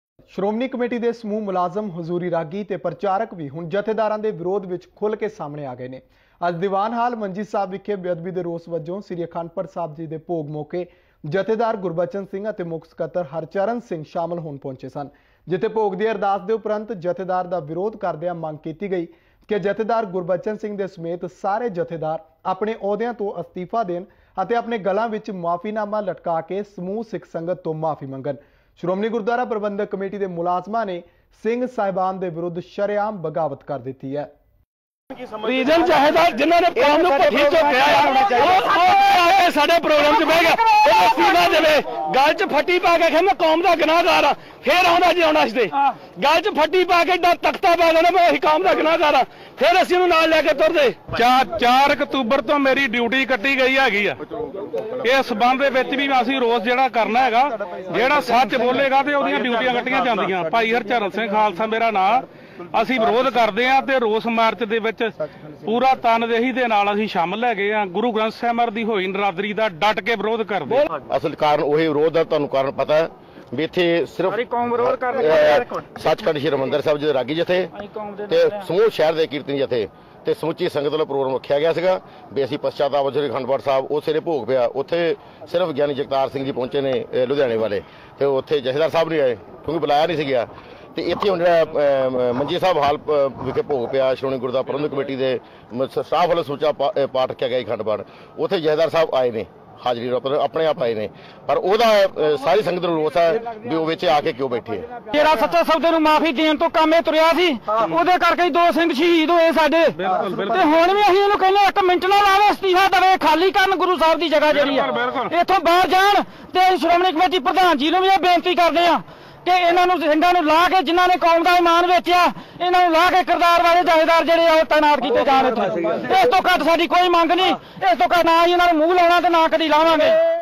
ਹੁਣ ਸ਼੍ਰੋਮਣੀ ਕਮੇਟੀ ਮੈਂਬਰਾਂ (S.G.P.C) ਨੇ ਬੇਅਦਬੀ ਮਾਮਲੇ ਤੇ ਕੀਤਾ ਜਥੇਦਾਰ ਗਿਆਨੀ ਗੁਰਬਚਨ ਸਿੰਘ ਦਾ ਵਿਰੋਧ ।
ਅਮ੍ਰਿਤਸਰ ਦਿਵਾਨ ਹਾਲ ਵਿਚੋਂ ਬਾਹਰ ਕੱਢਿਆ।